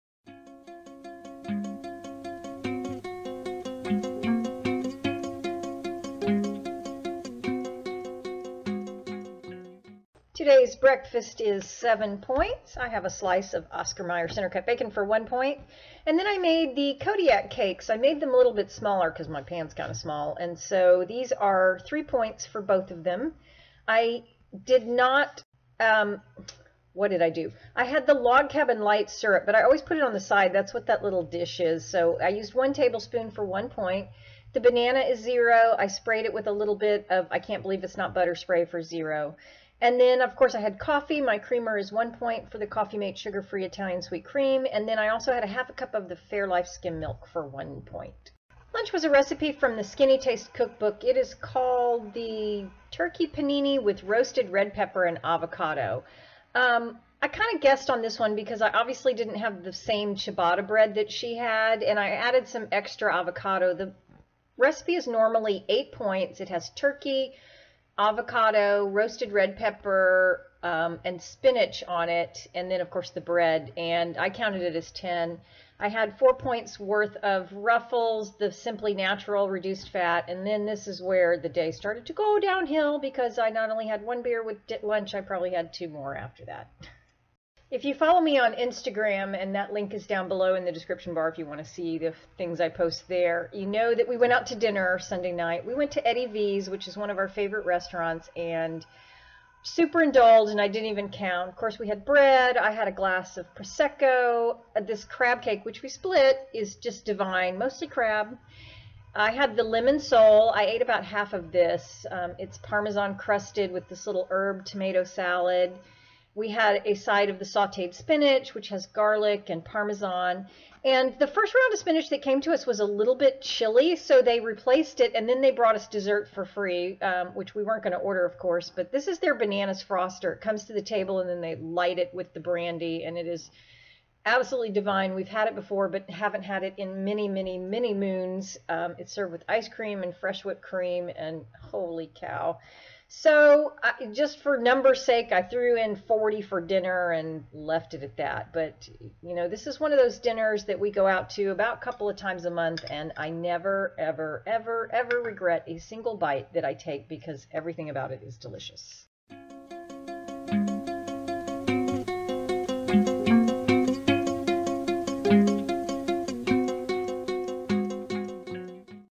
Everyday Smart Points - May 29 2016
Many of us with thyroid issues also struggle with weight. As a current member of Weight Watchers under the new Smart Points system, I thought it would be helpful for you to see what I eat on a

I would love to hear your comments, suggestions or requests!

Zero Point Soup Recipe There are lots of variations to this one, so customize to your desire. Just watch out for point-y add-ins like corn and pasta.

Any website or other resource mentioned is not paying me any money to make this video and they do not know that this video is being made. I mention them because they are part of my journey and I find them genuinely helpful.

From the Free Music Archive
CC